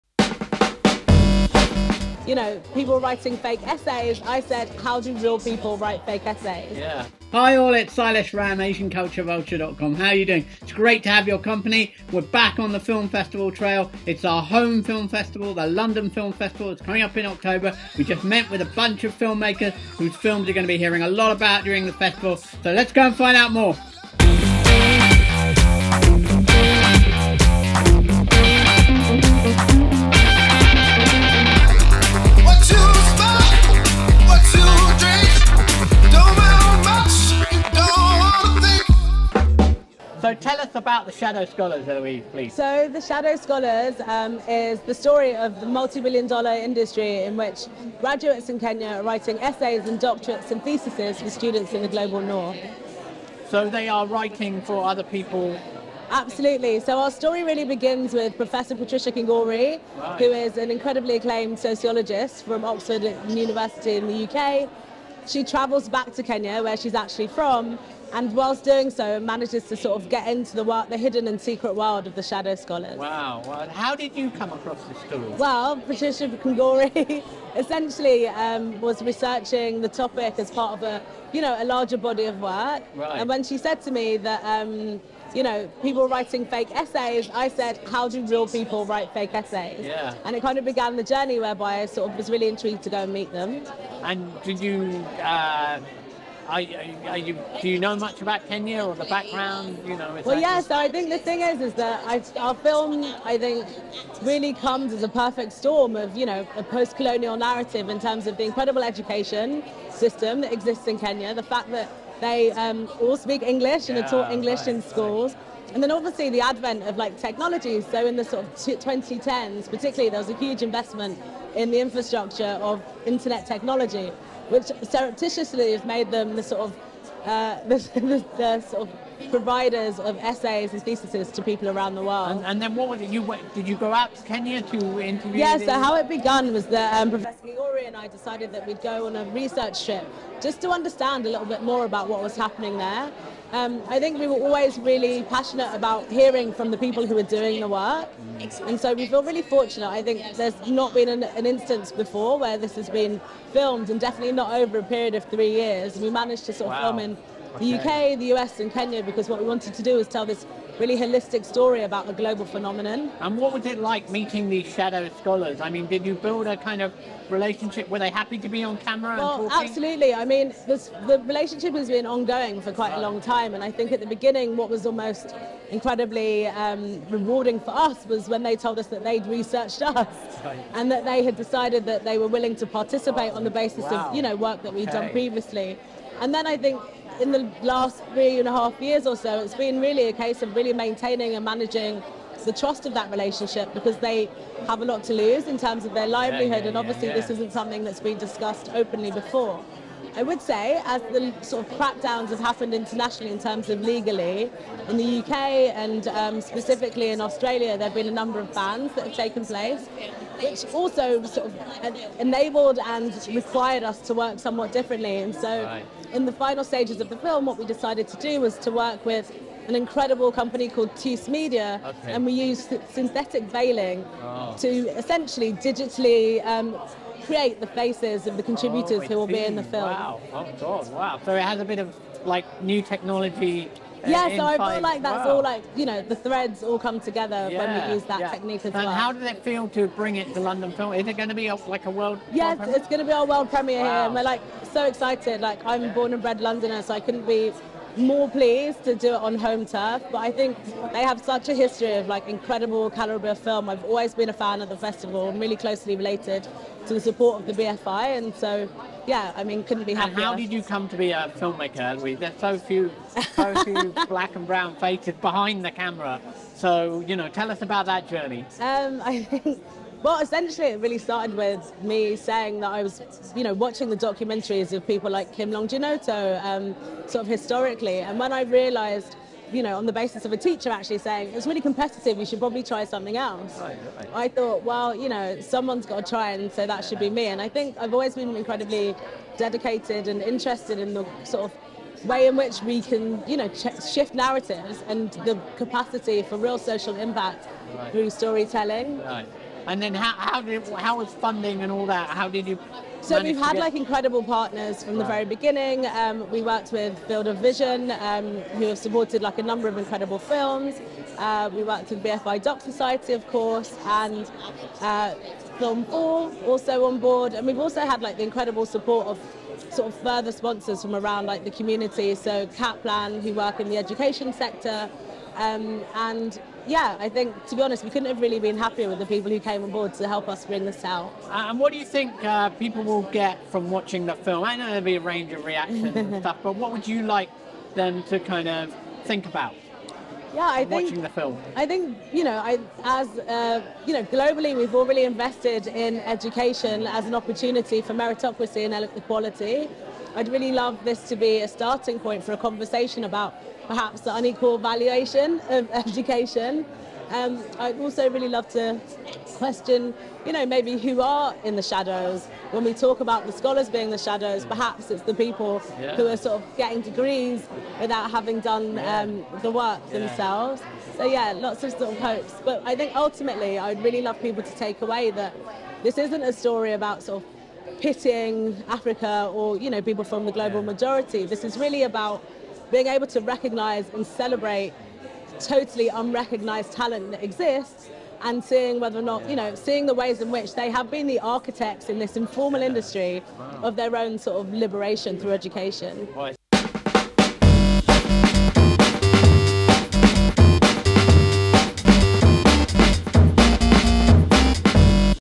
LFF 2024 - The Shadow Scholars doc - the murky world of Kenyans writing essays for rich students...
DIRECTOR Eloise King speaks to acv's Sailesh Ram about the film she has made about students in Kenya, in the global South, writing essays for students in the global North, for money...
The documentary starts with Oxford University Professor Kingori going back to Kenya, and her own roots, and uncovering what is a huge multi-billion industry which has talented students unable to fulfil their potential in the global south...

Presenter/producer: Sailesh Ram
Editing/camera: Harry Clegg